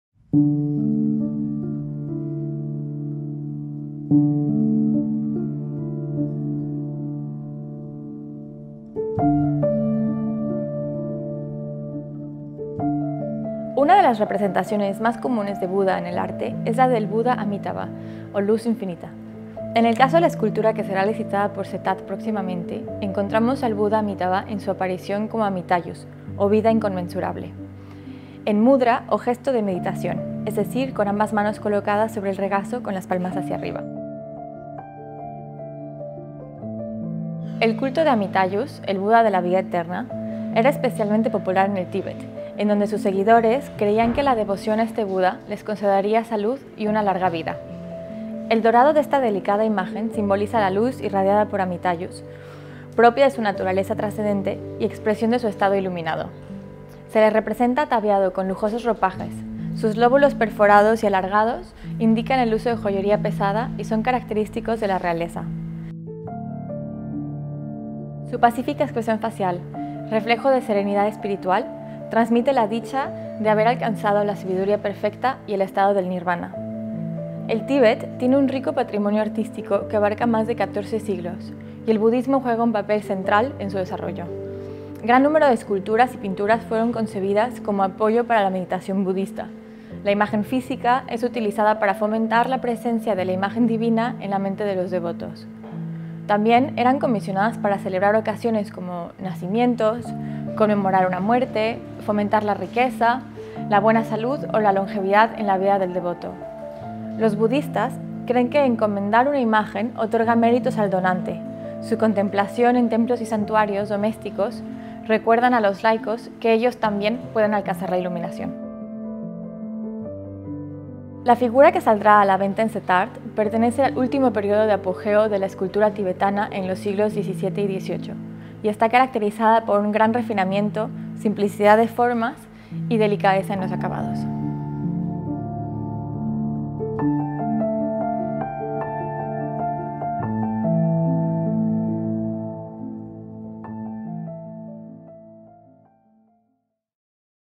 Buda Amitayus, Tibet, siglo XVII-XVIII.
Visita para encontrar más lotes como este.

Las figuras antropomórficas de Buda tienen su origen en el norte de la India y los actuales países de Afghanistan y Pakistan en el siglo I d.C. Durante los siglos posteriores, las representaciones de Buda y sus enseñanzas se esparcen en prácticamente cada cultura desde Afganistan hasta Japón y son objetos de contemplación, meditación y veneración en hogares y templos.

Las esculturas de Buda, realizadas en una infinidad de estilos y materiales, están cargadas con símbolos de sus enseñanzas de compasión y sabiduría: desde los pliegues de sus ropajes y el pecho descubierto, hasta los distintos gestos con las manos llamados mudras.